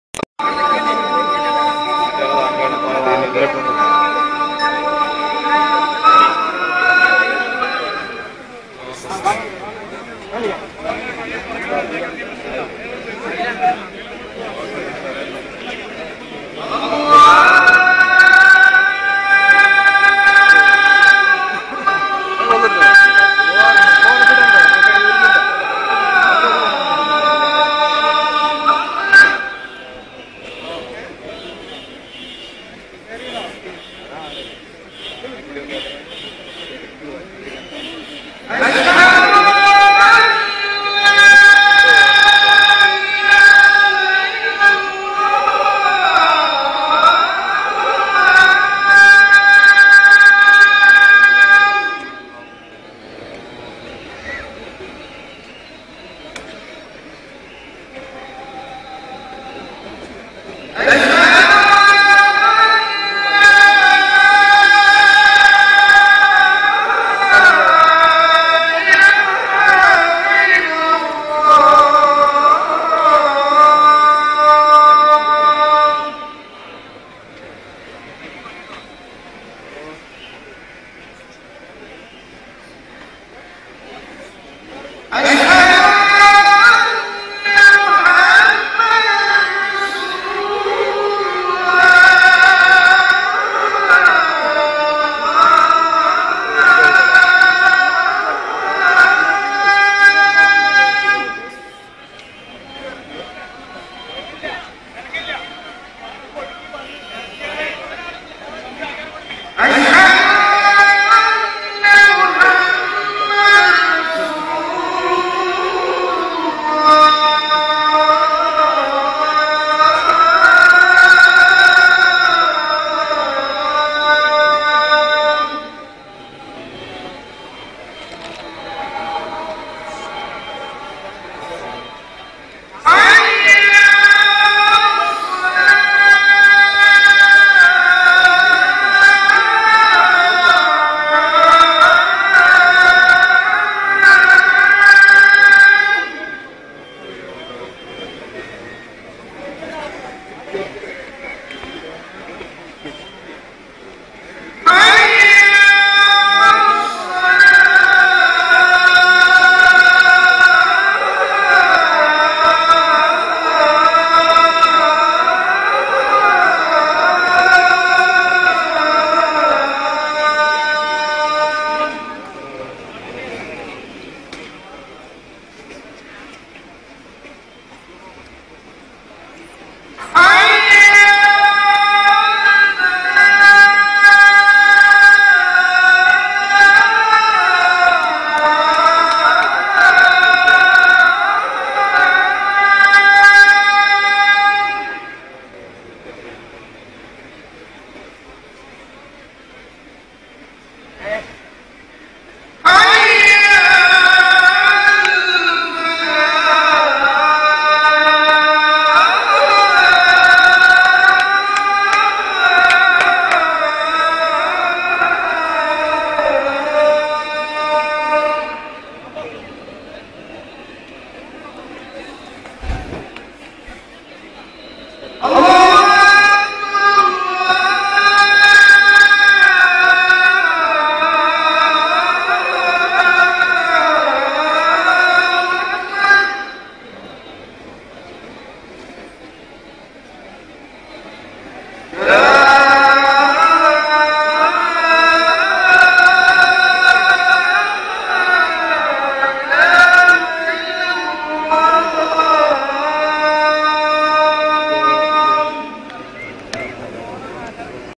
آذان المغرب من جامعة مركز الثقافة السنية كيرالا، الهند
آذان المغرب 6-1-2013 من جامعة مركز الثقافة السنية. بولاية كيرلا الهند
Magrib adan, Imam Masjid An Nabavi,Madina, form Jamia Markazu Saqafathi Sunniya. 06/01/2013
See his adan from Madina